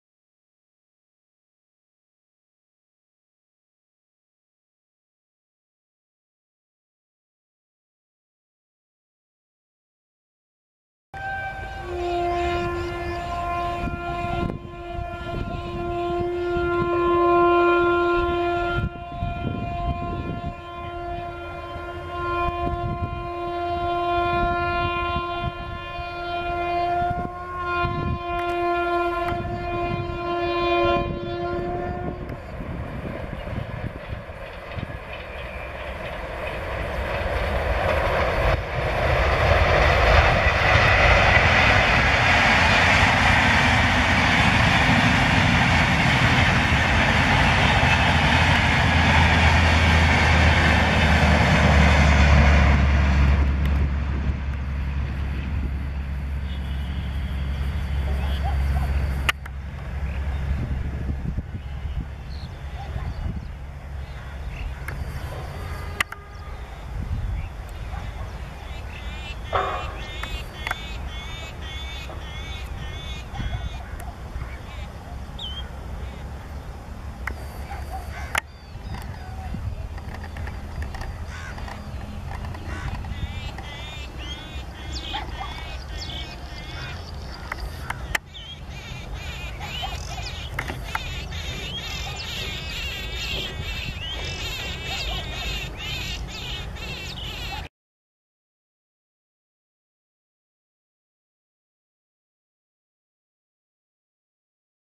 Indian Railways - Kanpur Shatabdi Blasts Past Beautiful Wheat Fields...:)
New Delhi - Kanpur Shatabdi is the Fastest Train Of Indian Railways in terms of AVG.Speed and has an avg of 92Kmph . It covers the distance of 441 Km in 4 Hours and 45 Minutes . It Consists of a LHB Rake and has an MPS of 130 KmPh.